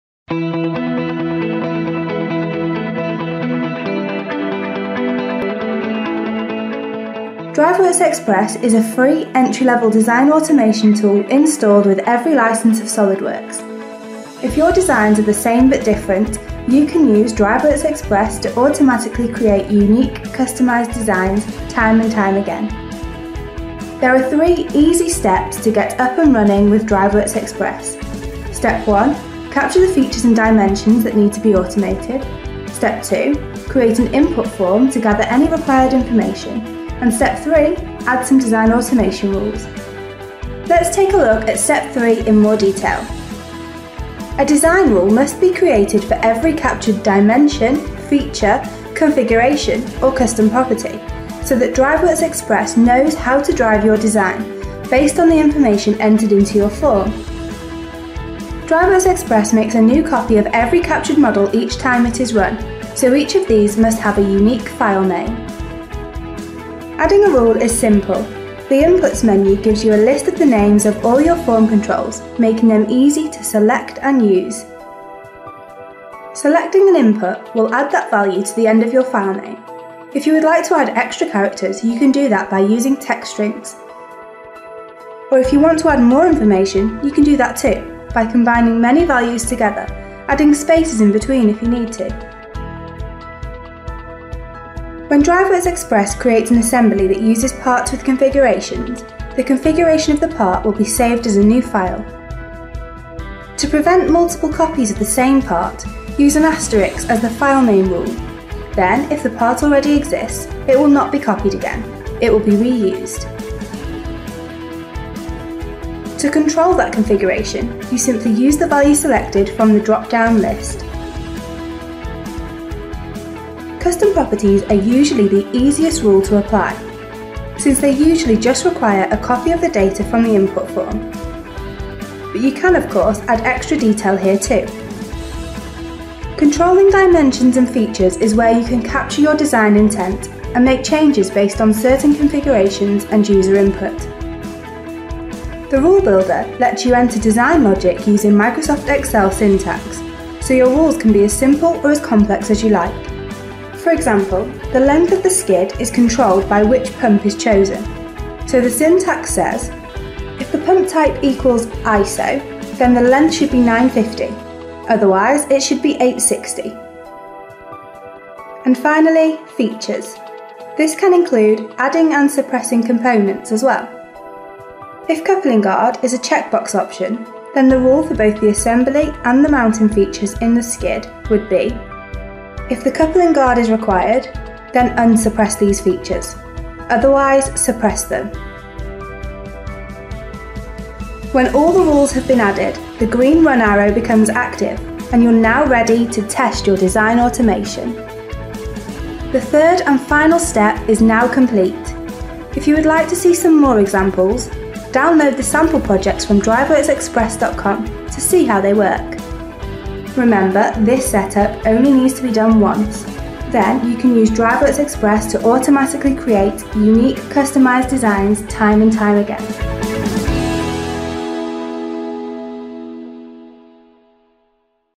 Setting Up DriveWorksXpress - Step 3: Adding Rules
There are 3 easy steps to get up and running with DriveWorksXpress:
1. Capture SOLIDWORKS Models
2. Create a Form
3. Add Design Automation Rules

In this video, we will take a look at the final step in setting up DriveWorksXpress - how to add Design Automation Rules.